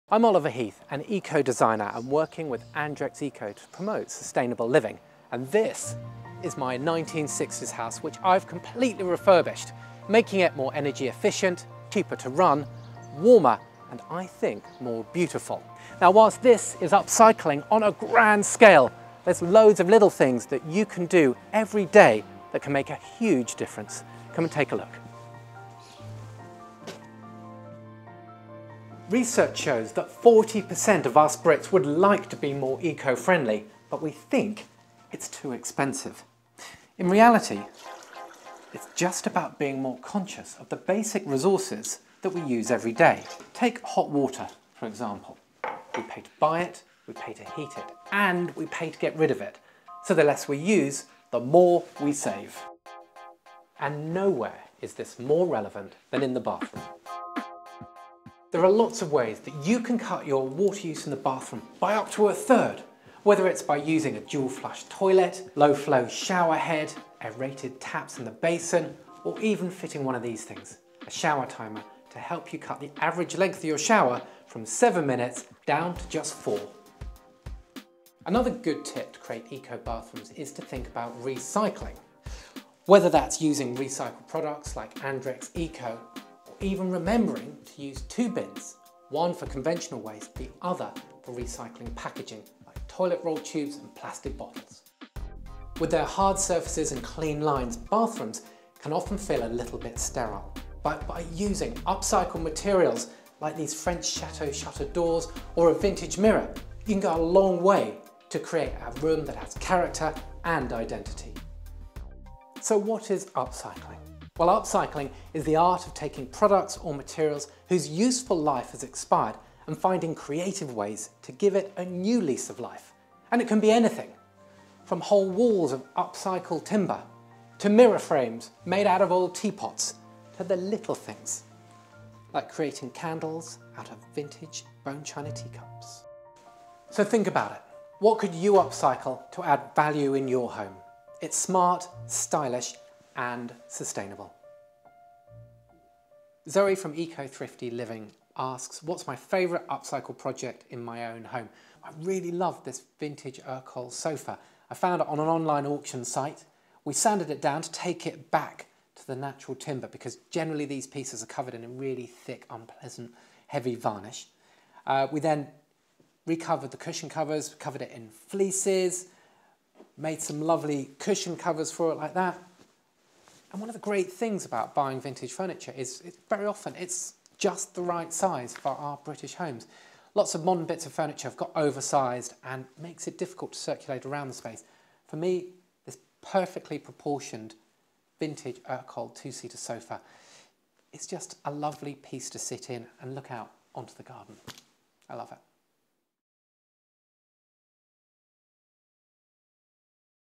Zoe from Eco Thrifty Living has a question for Oliver Heath
Zoe from Eco Thrifty Living asks "What is your favourite upcyled project in your own home?"

Eco design exper Oliver Heath, has teamed up with Andrex Eco to share his tops tips on sustainable living. For more information about the little things you can do to be more sustainable